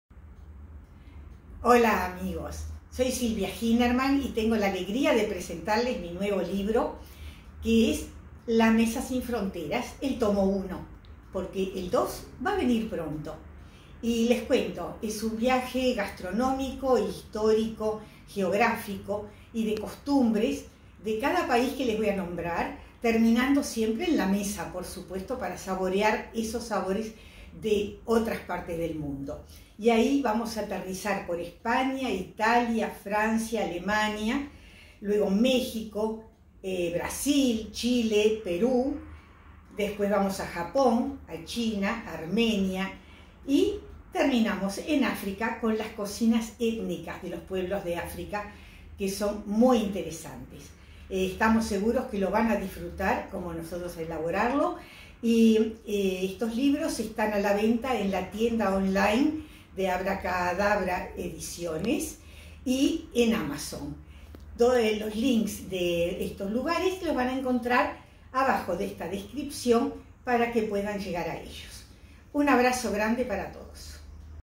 La Mesa sin Fronteras (Volumen 1), de Silvia Ginerman
Este libro es para gustar. Hallarás en él aromas y sabores, que se dibujan en diferentes paisajes que hemos elegido para este primer tomo: costas, valles y montañas, y sus mesas tendidas.

En este primer volumen de la serie, la autora viaja contándote recetas típicas. Empieza por la Europa Mediterránea, España, Italia, Francia, y Alemania. Recorre después sabores de México, Brasil, Chile y Perú. Y de Asia nos trajo recetas de Japón, China y Armenia, para completar el paseo en Marruecos, el África Subsahariana y Magadascar.

Un valor agregado no menor: salvo en algún caso donde lo importante era la anécdota (como en la sopa de hipopótamo africana, digamos) la autora adoptó las recetas de modo en que se las pueda preparar de modo completamente Kashér.

En casi todas las plataformas mientras se pueda: @danielginerman